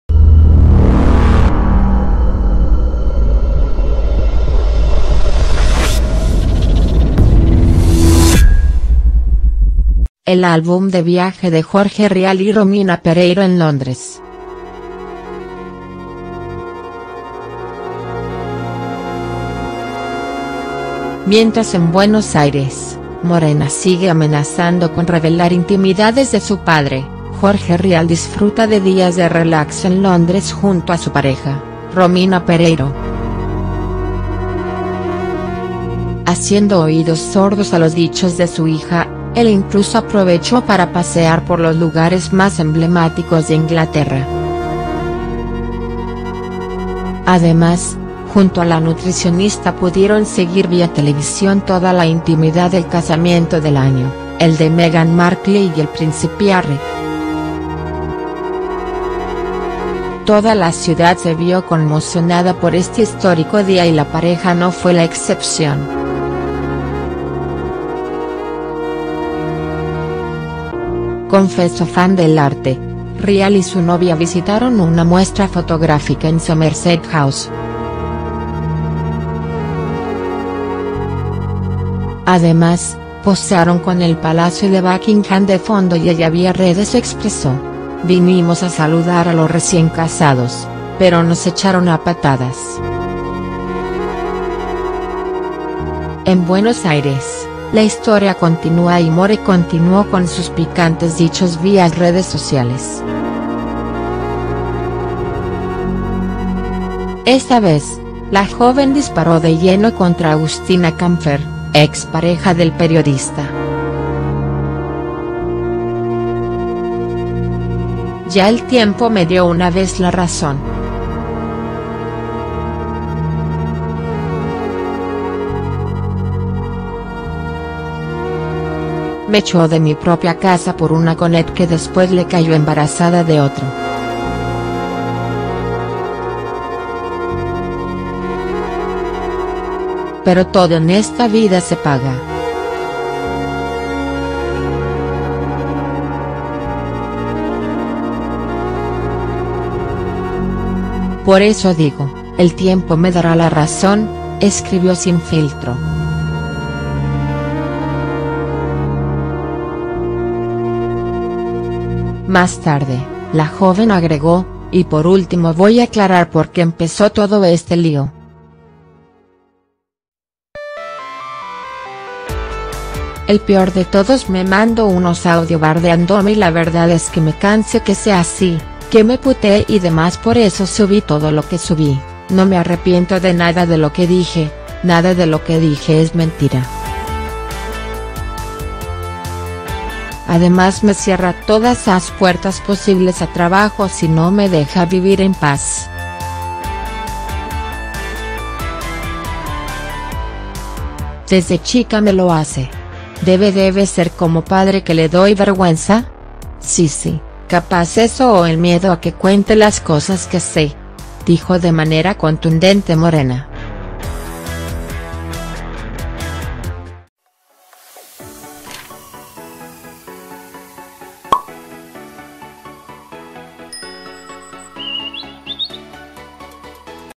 El álbum de viaje de Jorge Rial y Romina Pereiro en Londres
Mientras en Buenos Aires, Morena amenaza con revelar intimidades de su padre, el conductor disfruta de días de relax en pareja.
► With the above criteria, if th